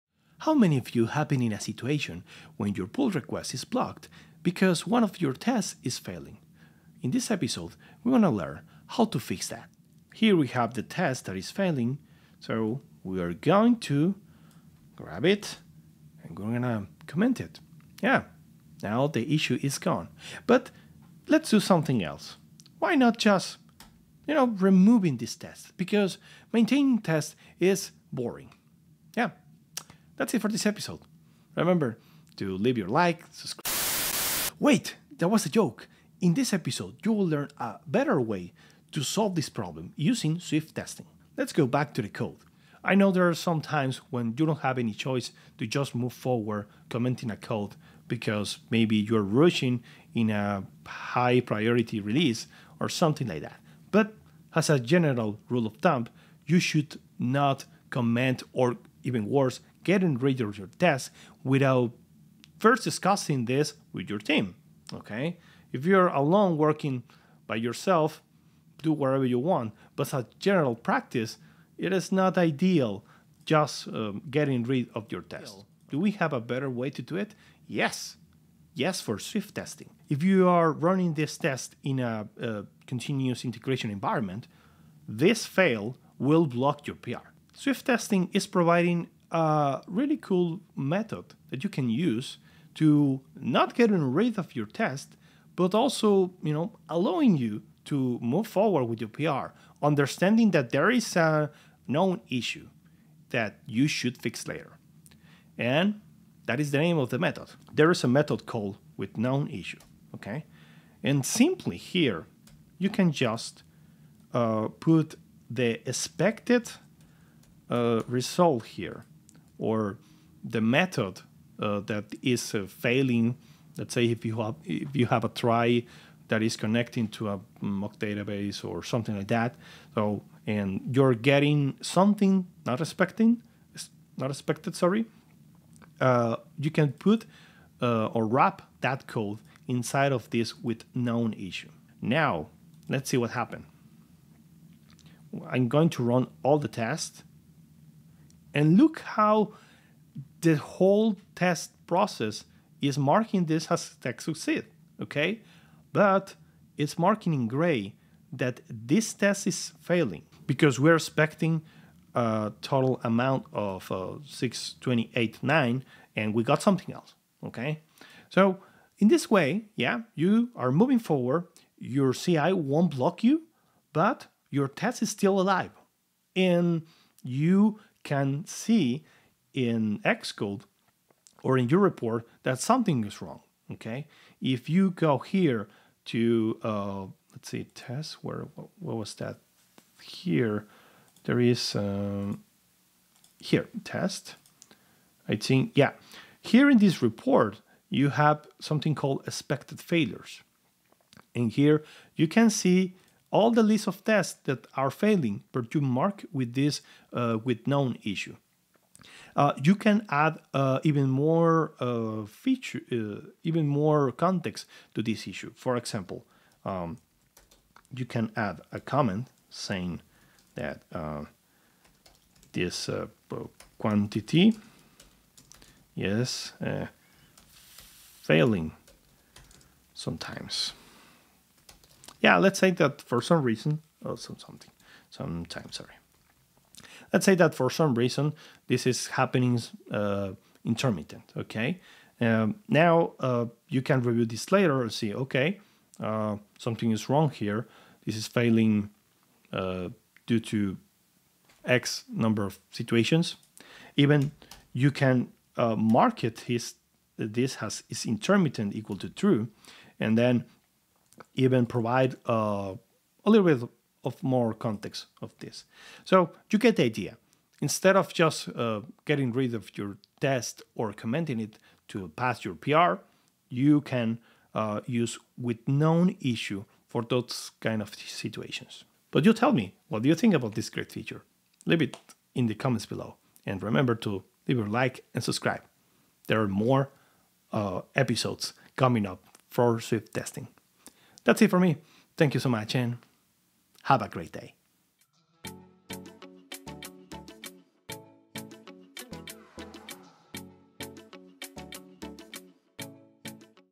Mastering Swift Testing: Managing Test Failures with withKnownIssue
In this video, we explore the powerful withKnownIssue method from the Swift Testing framework. When working on real projects, ensuring a stable and robust app through thorough testing is crucial. Continuous Integration (CI) environments help by running a series of checks, including unit tests, before merging pull requests.

However, a single failing test can halt the entire CI process, which can be frustrating, especially when you have urgent releases. The common workaround is to comment out or remove the failing tests, but this can lead to bad habits and decreased test coverage over time.

Swift Testing introduces a better solution: the withKnownIssue method. This method allows you to wrap failing expectations, preventing them from causing CI pipeline failures. Instead of marking these tests as failed, Xcode categorizes them under "expected failures," which can be addressed later.

Content:
0:00 - Intro
0:42 - withKnownIssue
3:28 - withKnownIssue with parameters
4:39 - Outro